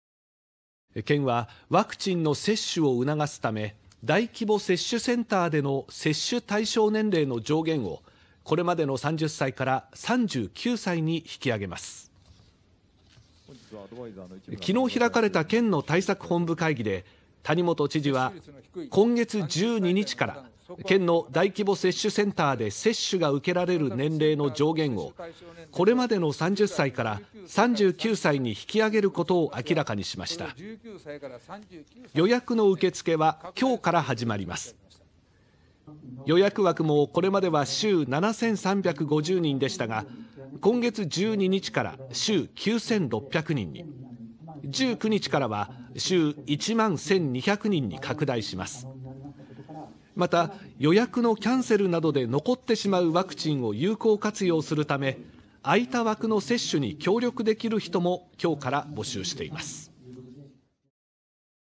石川県の大規模接種 対象を３９歳まで拡大 2021.9.10放送
石川県はワクチンの接種を促進するため、大規模接種センターで接種を受けられる対象年齢の上限をこれまでの３０歳から３９歳に引き上げます。９日に開かれた県の対策本部会議で、谷本知事は今月１２日から、県の大規模接種センターで接種を受けられる対象年齢の上限をこれまでの３０歳から３９歳に引き上げることを明らかにしました。予約の受付は１０日から始まります。予約枠もこれまでは週７３５０人でしたが、今月１２日から週９６００人に拡大し、今月１９日からは週１万１２００人に拡大します。また、予約のキャンセルなどで残るワクチンを有効活用するため、空いた枠の接種に協力できる人も１０日から募集します。

月～金　18:15~

11:41~　／　20:54~